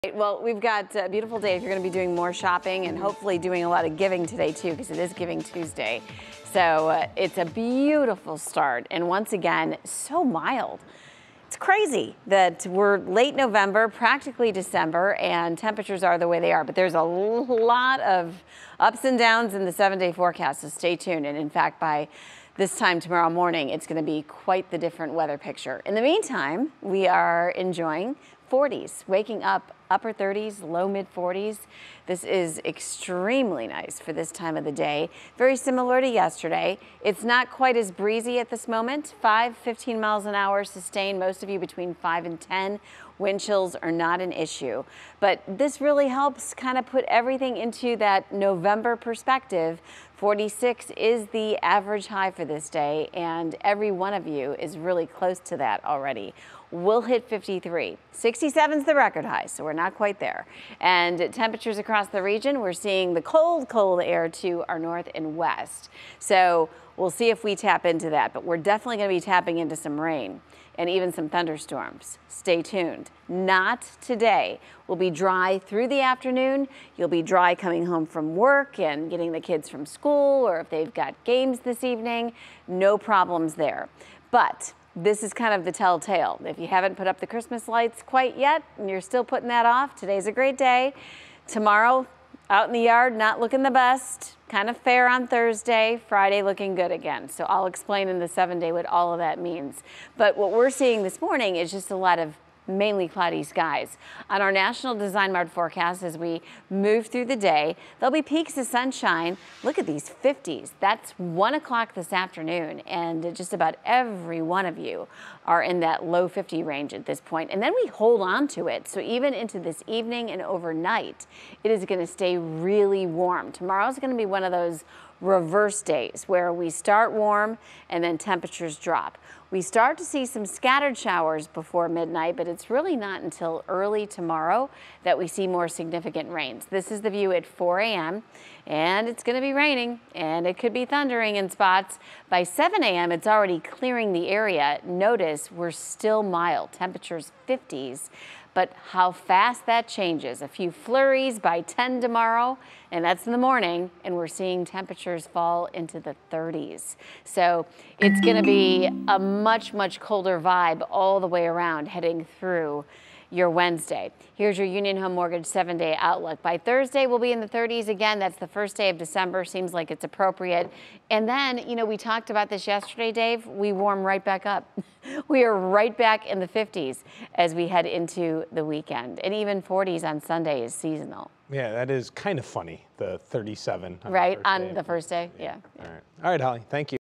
Another round of cold air on the way: Cleveland weather forecast for November 29, 2022
While temperatures will top 50 degrees today with mostly cloudy conditions, your Cleveland weather forecast shows some big changes are in store as much colder air arrives by Wednesday. As rain moves through the region early Wednesday morning, the temps will tumble to the upper 30s.

3News' Hollie Strano has the hour-by-hour details in her morning weather forecast for Tuesday, November 29, 2022. See the extended weather forecast